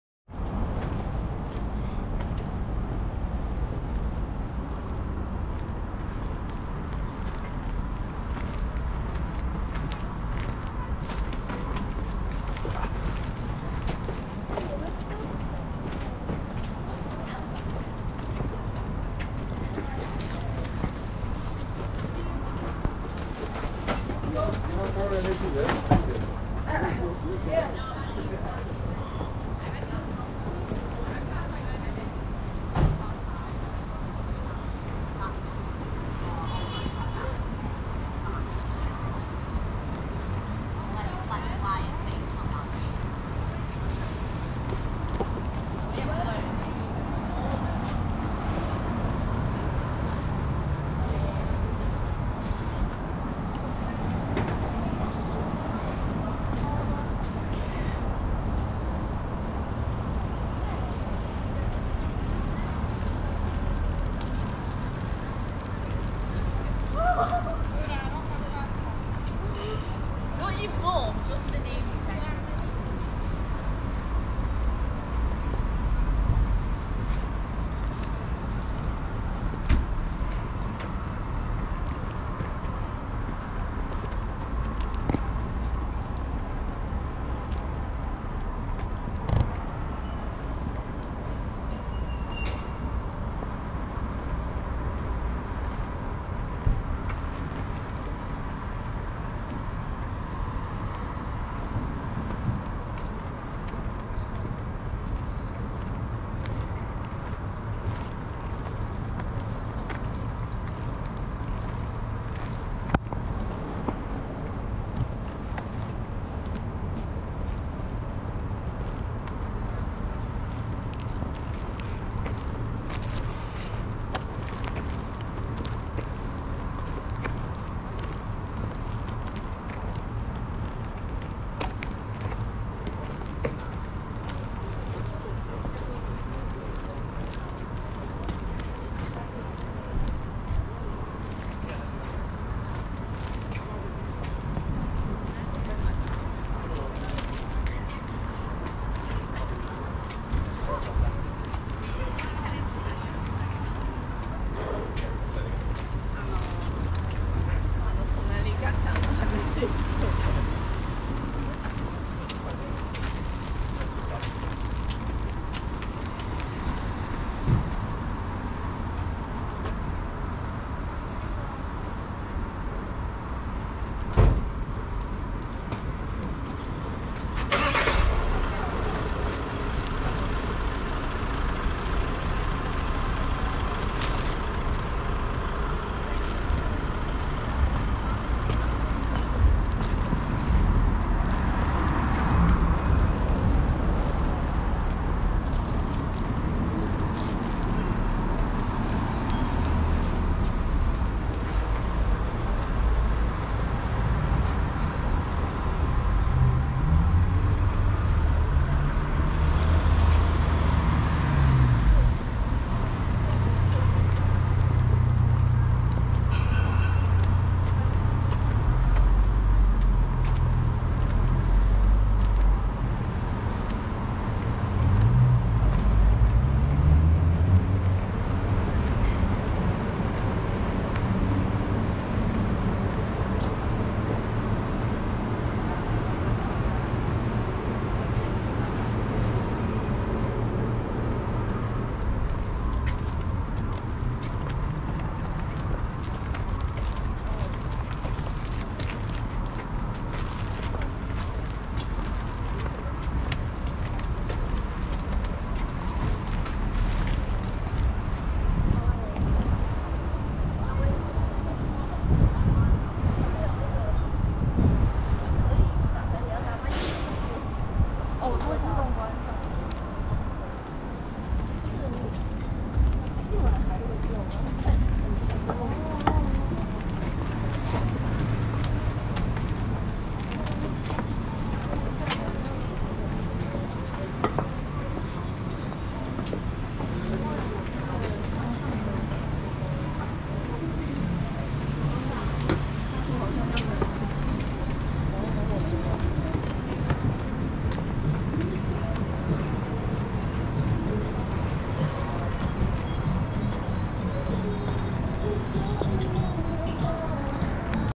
MANCHESTER CHINATOWN WALK - EARLY EVENING 4K Walking tour :) Near Manchester University
This mini walk takes in the lovely China Town area of Manchester.

I am also uploading a link to a walk in the same area at night to see the contrast.